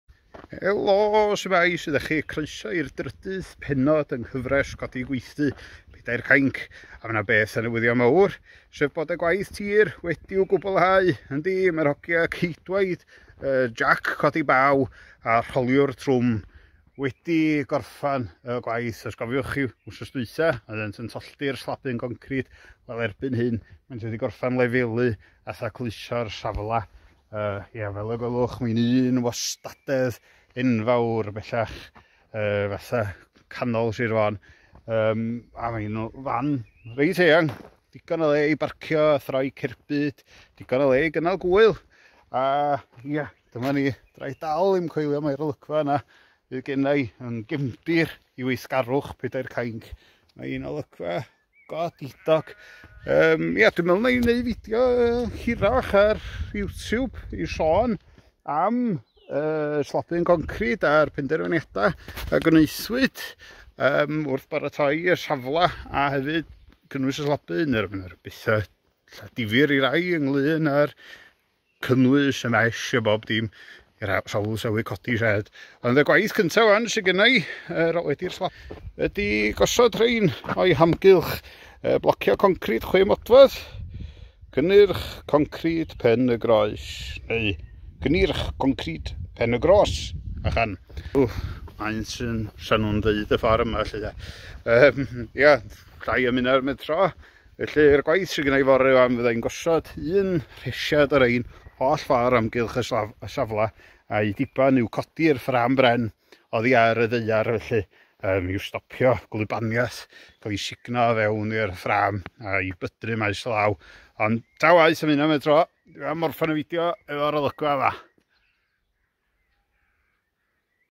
3 - Y gwaith daear wedi ei gwblhau [Gweithdy Pedair Cainc]
Wel, mae'r cam mawr cyntaf adeiladu gweithdy Pedair Cainc wedi ei gwblhau! Ydi, mae ceidwaid y Jac Codi Baw wedi rhoi taw ar y peiriant a'i throi am adref.

Tolltwyd y concrit yr wythnos diwethaf, ac mae hwnnw'n sychu'n dda ac yn ddigon cryf i gerdded drosto a gwneud gwaith ysgafn. Gymrith fwy neu lai fis i sychu a chaledu'n llawn.

Mae'r slabyn yn 12.5m o hyd a 10m o led. Ac i'r gîcs y byd concrit, efo mesh 10mm yn ei ganol ac yn un C35 o ran ei gryfder (dwi'n fwy o hwyl dros beint) - mae'n ddigon cryf i ddal Powys.

O'i hamgylch, gosodwyd llwybr a wyneb parcio a throi i'r fan, a hwnnw fel Maes Sioe Môn.

Mae llwyth o waith wedi ei gwblhau gan yr hogiau, defnyddiwyd 180 tunnell o gerrig bras, a dros 100 tunnell o gerrig glan.

Rwan fod y safle'n ddistaw rhag dwrw'r peiriannau mawr, finnau fydd yn ymgymryd â'r gwaith adeiladu o fory ymlaen. Byddaf yn dechrau gosod blociau 6" o amgylch ymyl y slabyn erbyn diwedd yr wythnos, a gweithio ar y ffram bren wedi hynny.

Well, the first phase of Pedair Cainc's workshop build has been completed! The machines are no longer running and have returned to the yard.

The slab was poured last week, and has now hardened enough to walk on and do some light work. It will take around 28 days to fully cure.

It measures 12.5m x 10m, and for the concrete geeks out there, has a 10mm mesh and is of a C35 spec (I'm not that boring on a night out) and could hold the weight of Powys.

Surrounding, there is a path and a hard surface for parking and turning the van, and is as flat as Belgium.

Now the site's quiet, my work will now properly begin. The first job will be to lay 6" blocks around the edge, after which, I will begin to cut and prepare the timber frame.